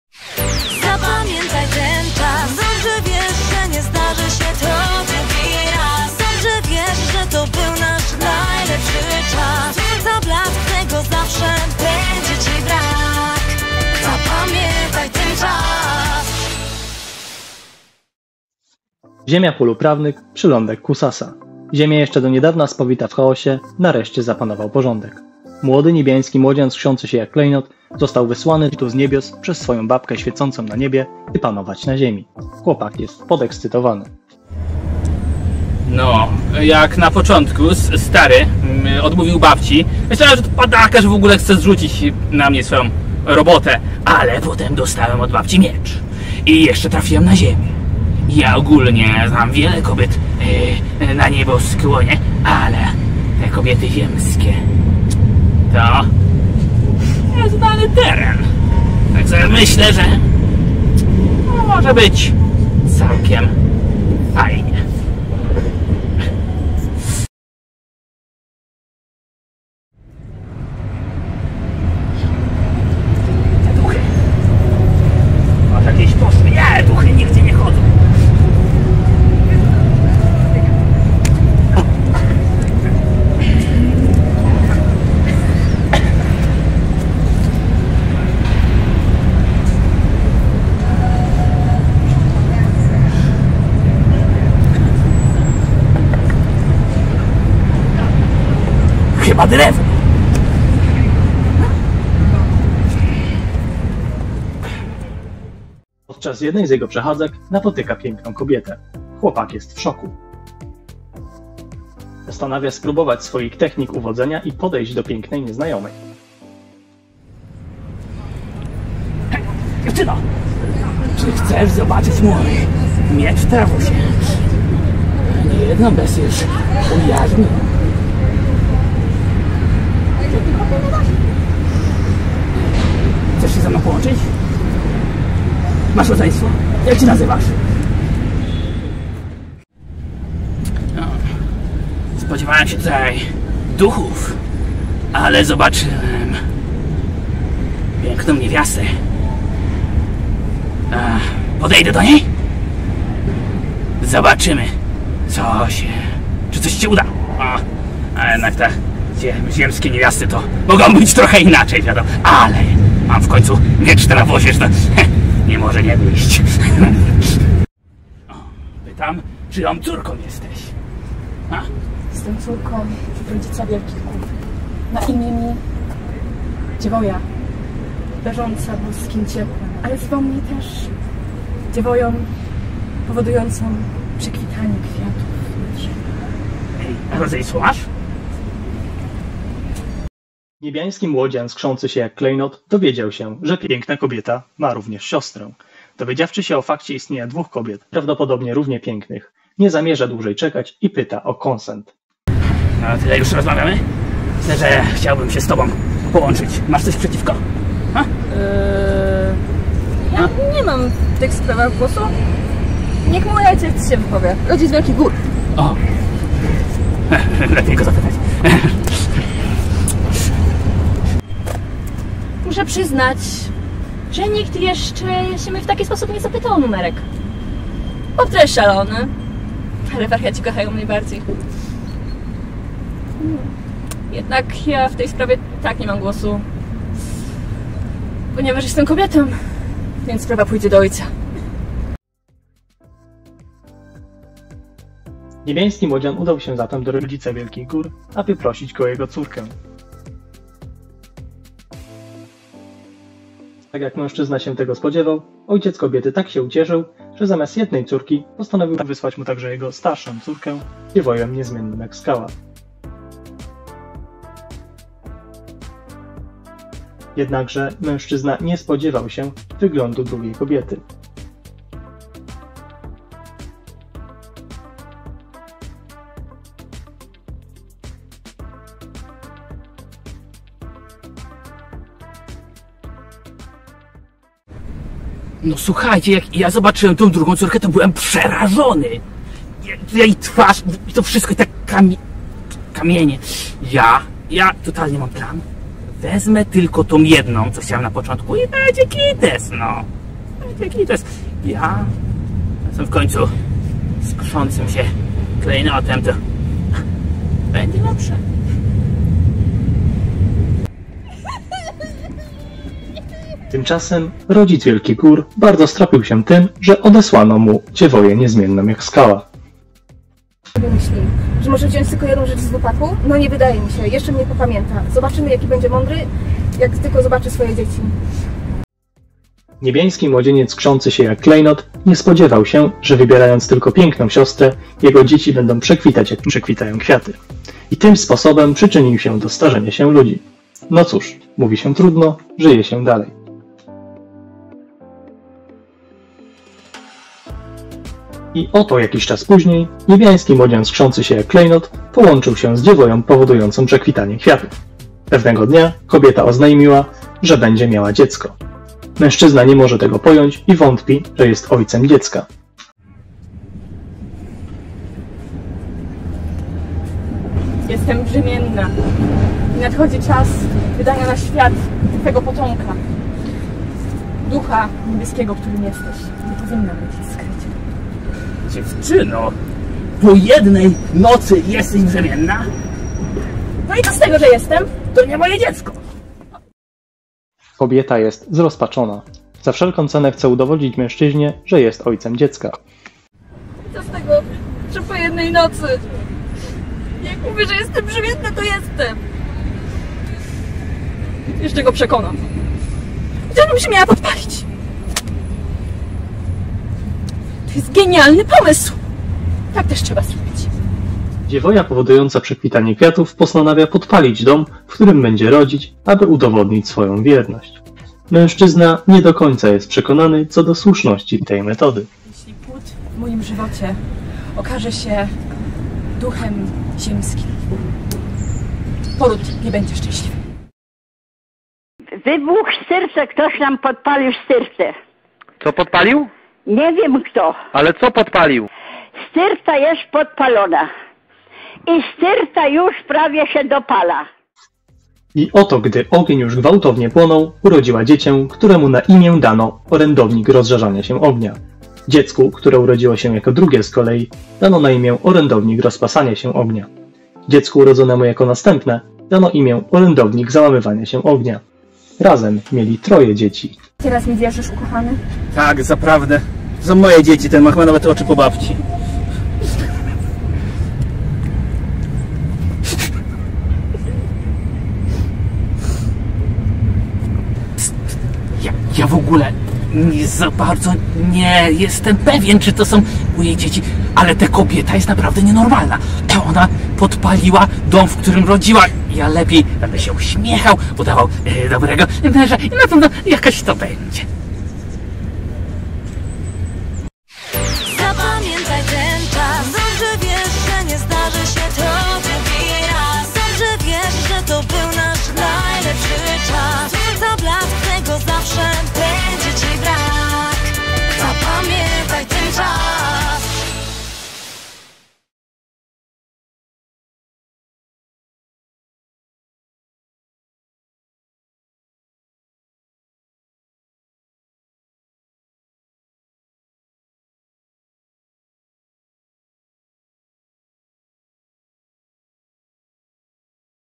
Pamiętniki z Kojiki odc 1
Pamiętniki z Kojiki Odc. 1: Odsłona interpretacji mitu z księgi Kojiki (710 n.e.), jaka została przygotowana przez studentów w ramach kursu Shinto. Filmowe dzieło przedstawia wydarzenia prowadzące do narodzin Japonii.
Występują: Alicja Mirek, Łukasz Lisek, Szymon Majewski, Edyta Harężlak, Liwia Cibis, Kinga Budzyńska, Tramwaje Krakowskie, Przypadkowi Przechodnie w rolach statystów.